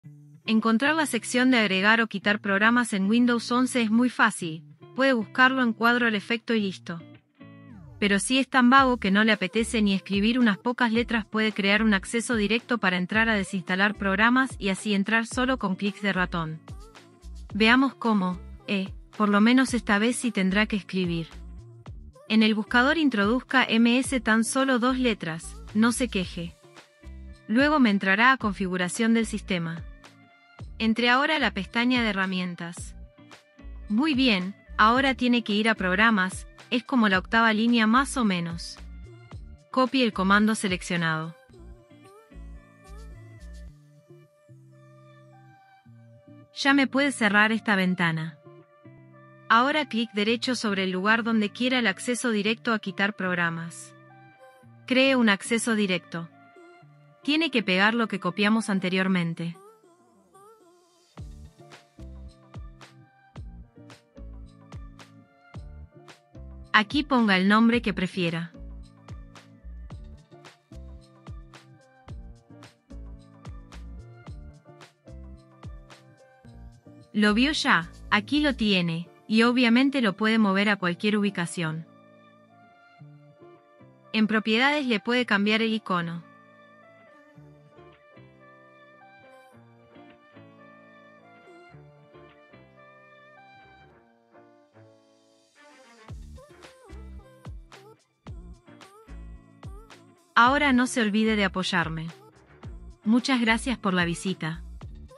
Cómo crear un Acceso Directo Para Desinstalar Programas y Aplicaciones🧑‍💻Windows 11
Enseño cómo crear un acceso directo para entrar a la sección de Agregar o quitar programas. El tutorial está hecho con sistema operativo Microsoft Windows 11 pero con Windows 10 el proceso es similar. Es una tutorial que se puede llevar a cabo en menos de 2 minutos y así ya podrá acceder tan solo con un clic a la parte desde donde puede desinstalar programas.

Es bastante fácil encontrar la sección para agregar o quitar programas en Windows 11, solo puedes buscarla en el cuadro para ese propósito, y eso es todo. Pero si le da pereza escribir algunas letras, puede crear un acceso directo para acceder al desinstalador de modo que esté a solo un clic del mouse. ¡Veamos cómo!

I teach how to create a shortcut to enter the Add/Remove Programs section. The tutorial is made with Microsoft Windows 11 operating system but with Windows 10 the process is similar. It is a tutorial that can be carried out in less than 2 minutes and so you can access with just one click to the part from where you can uninstall programs.  It's pretty easy to find the section for adding or removing programs in Windows 11, you can just look for it in the box for that purpose, and that's it. But if you are too lazy to type some letters, you can create a shortcut to access the uninstaller so that you are just a mouse click away. Let's see how!